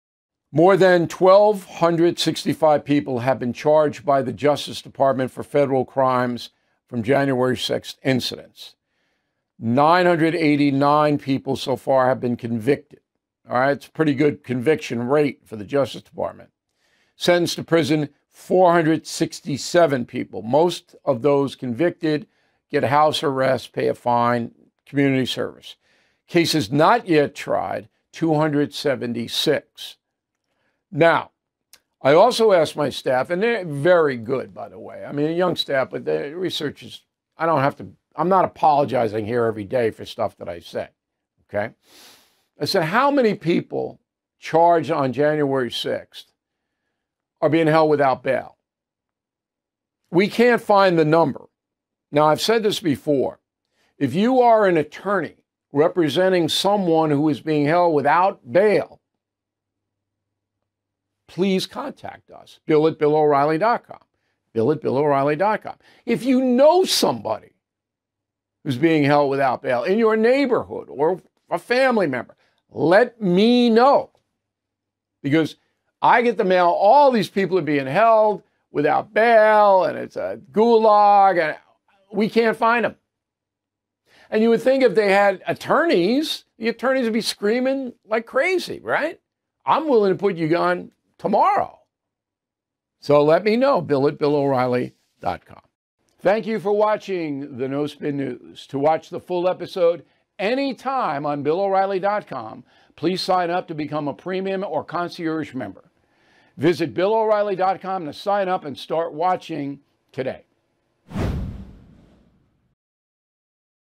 Updates on January 6th Arrests, Convictions, and Ongoing Cases with Bill O'Reilly
Bill reports on the status of January 6th arrests, convictions, and open cases.